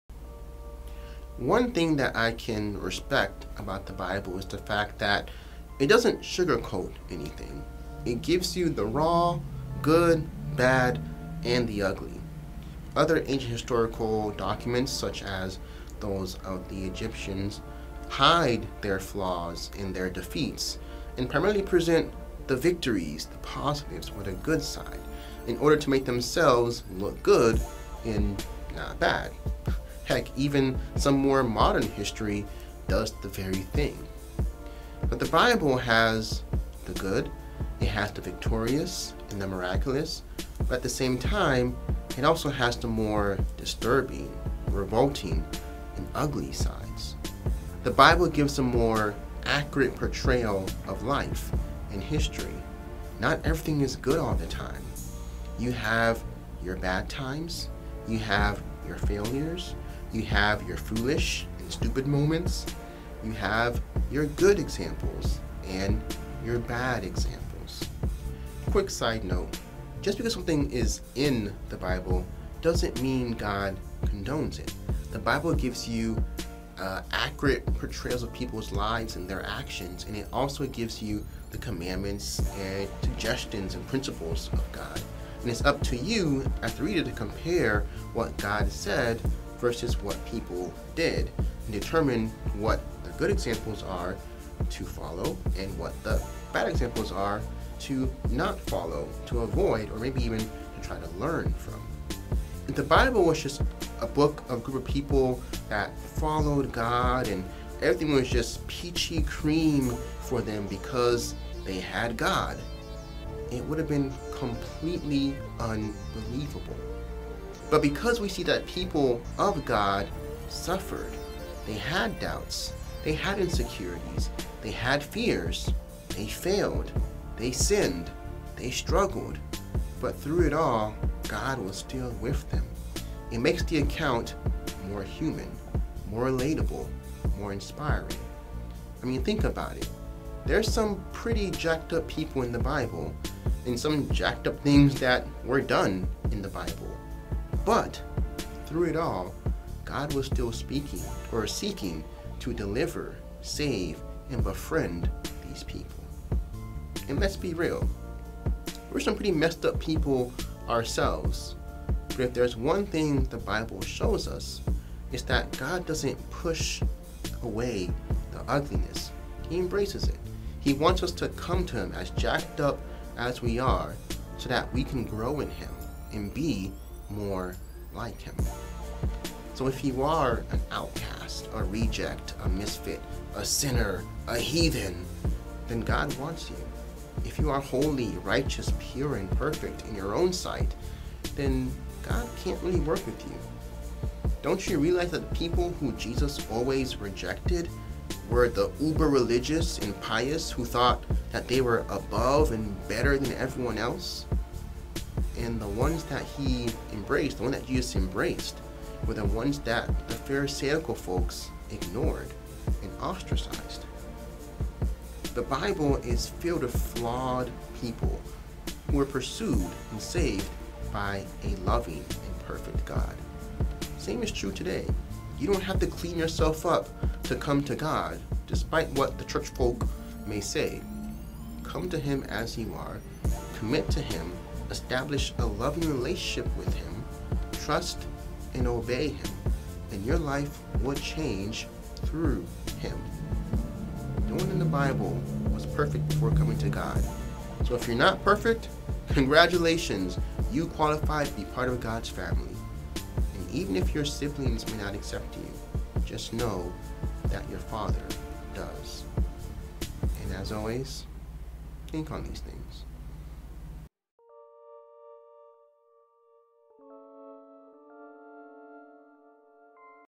The Bible is Ugly
The Bible is real...it gives you the good, the bad, and the ugly.
But thankfully, through it all, we have a good God who still desires to save us and have relationship with us.

Music Produced by:
Mike Kalombo
God Bless! :)